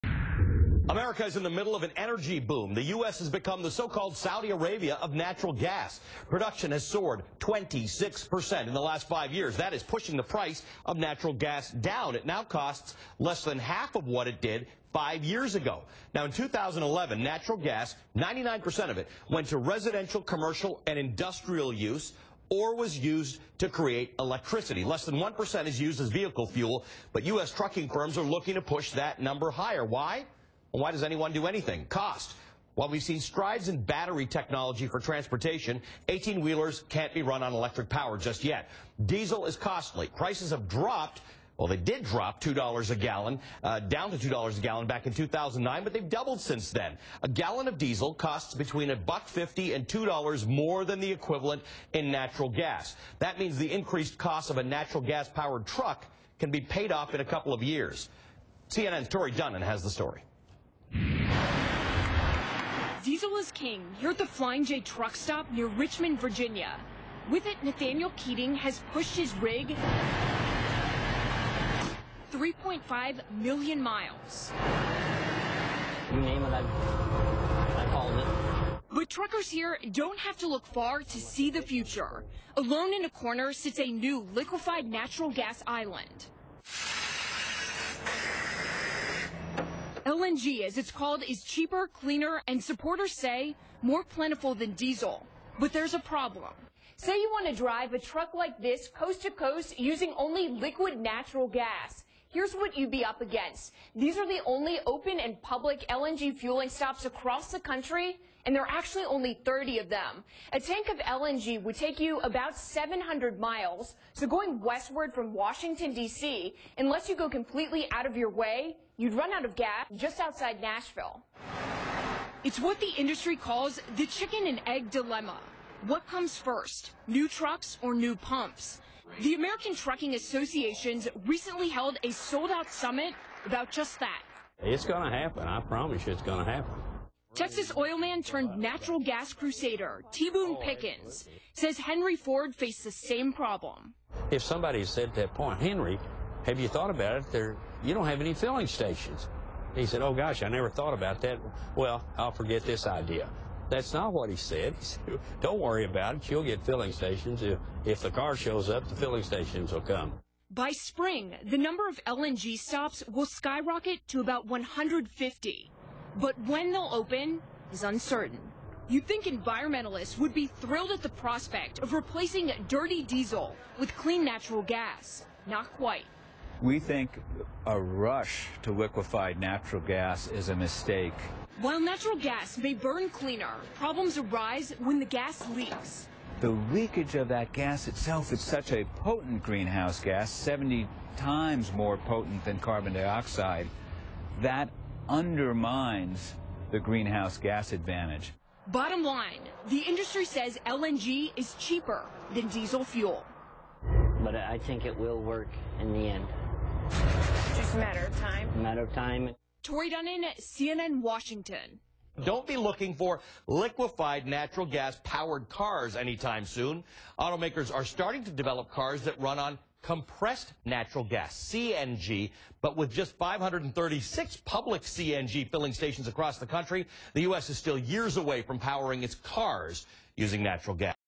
CNN on Natural Gas Trucking
A report on natural gas and trucking features ATA's Natural Gas in Trucking Summit.